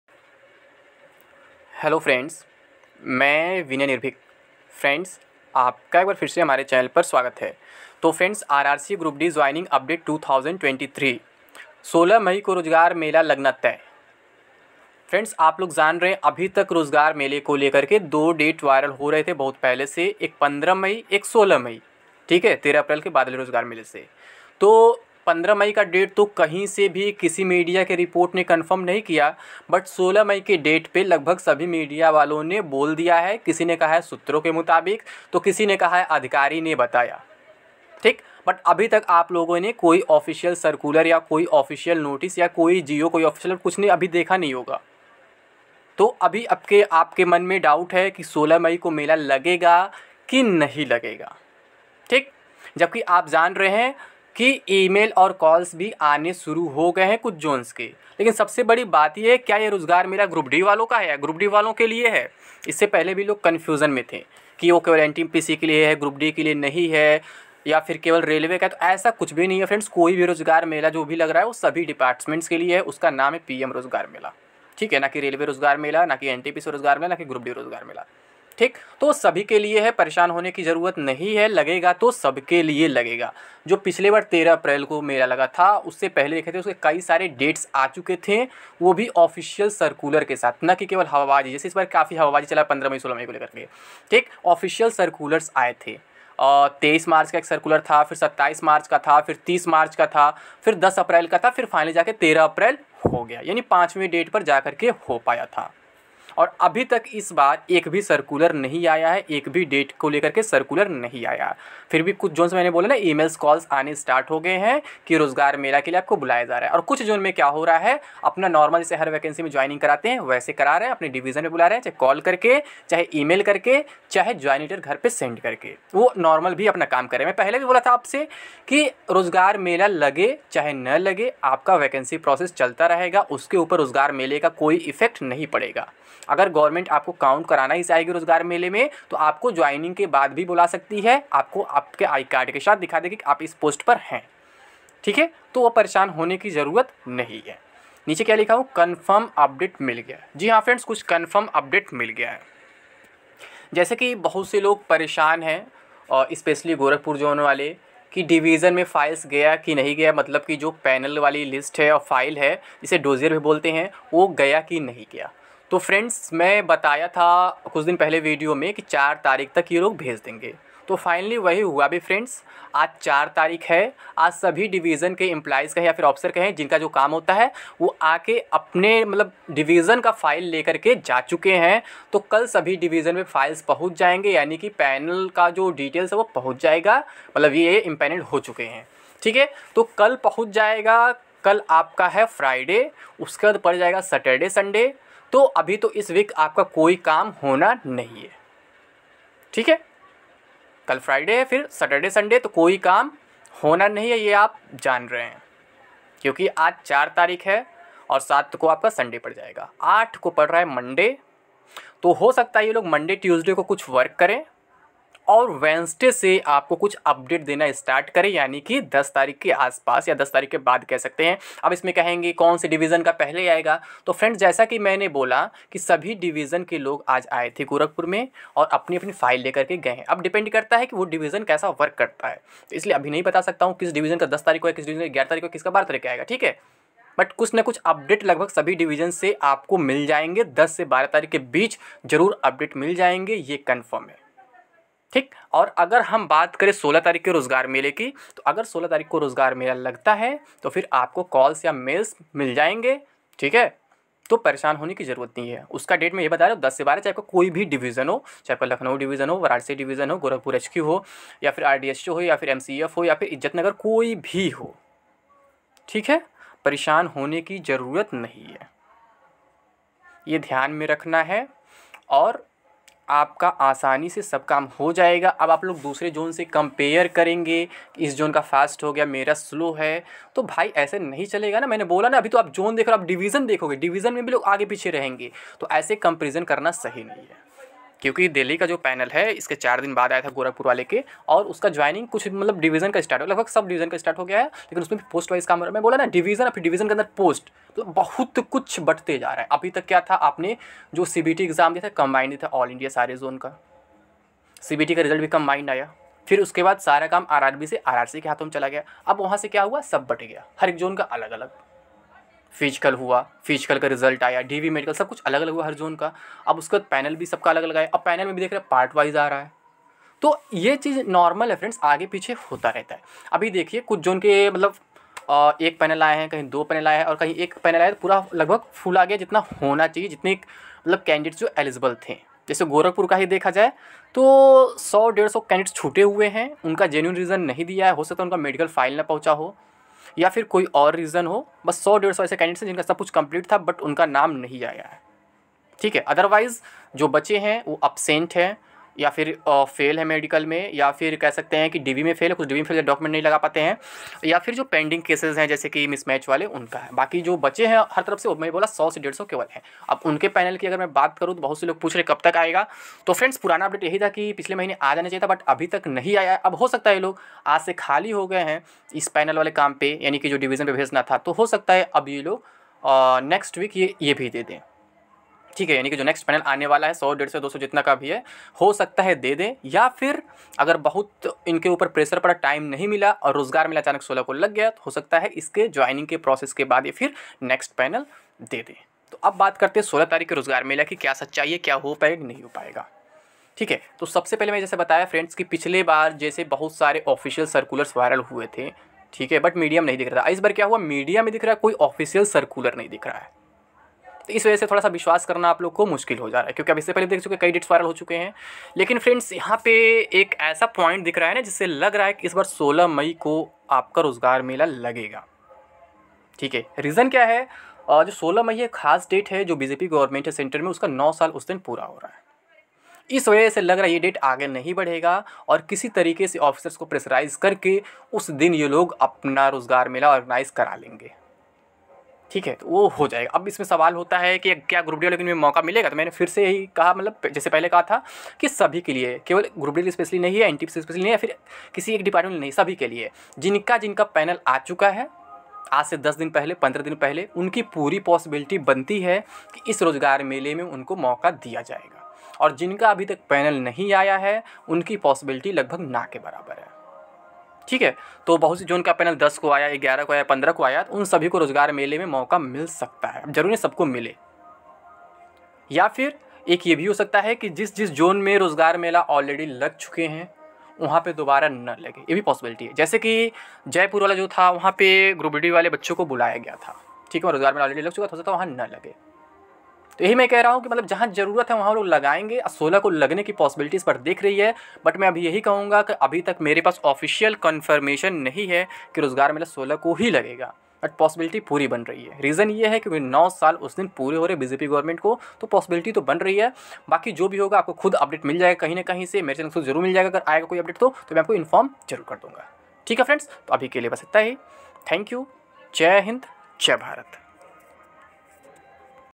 16 मई को रोजगार मेला लगना तय ? RRC Group D Joining Update / PM Rojgar Mela 2023 /
कृपया वीडियो अंत तक देखें और अपना Doubt हमेशा के लिए दूर करें !

up lekhpal news,
up lekhpal result update,